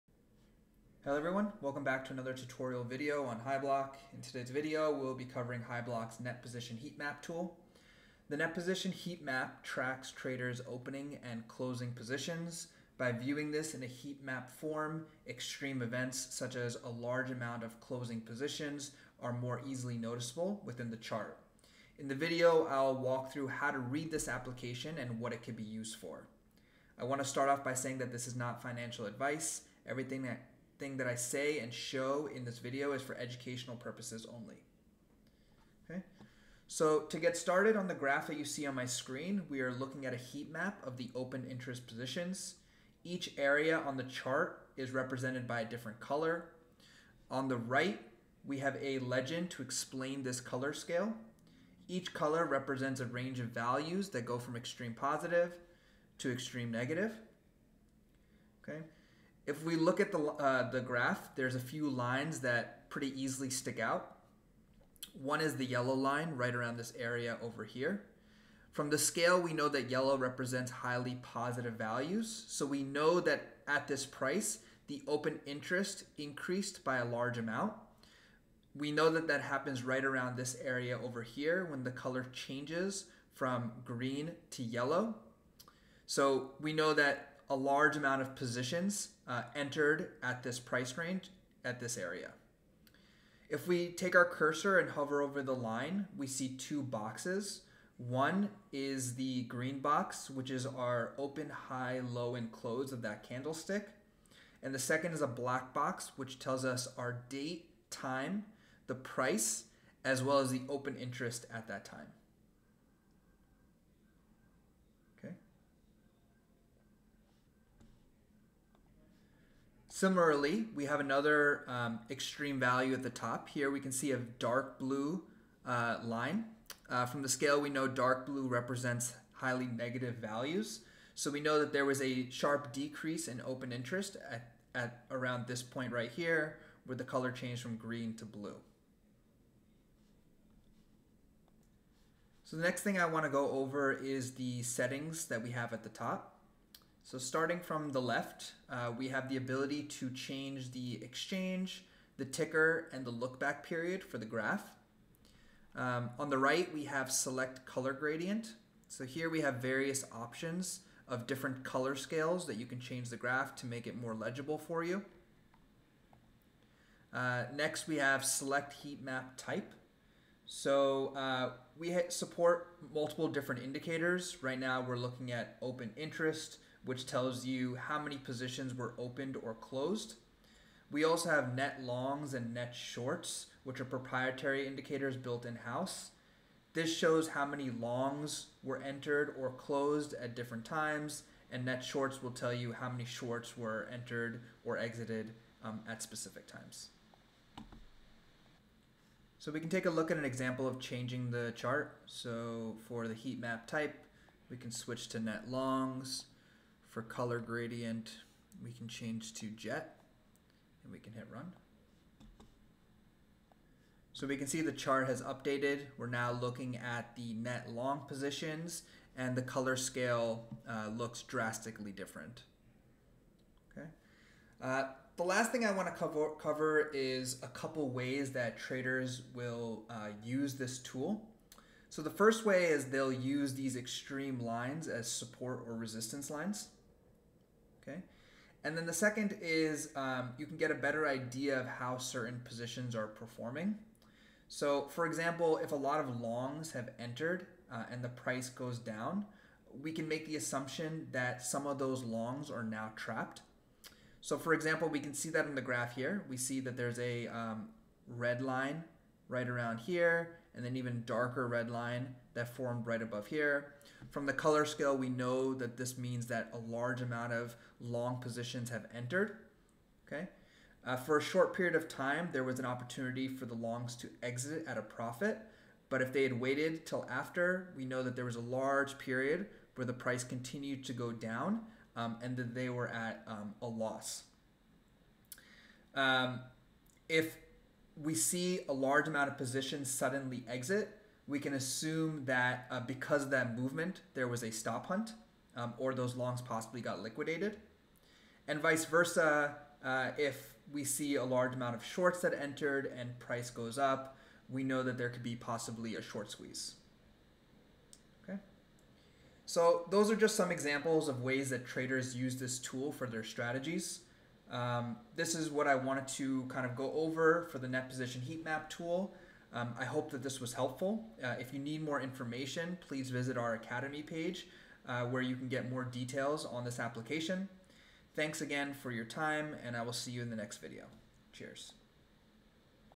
Hyblock Academy: Net Position Heatmap
This video is an overview of Hyblock's Net Position Heatmap Tool.

Hyblock is a research platform focused primarily on the world of cryptocurrency trading. Our product provide's unique and detailed data and analytics to assist crypto traders with research and building strategies.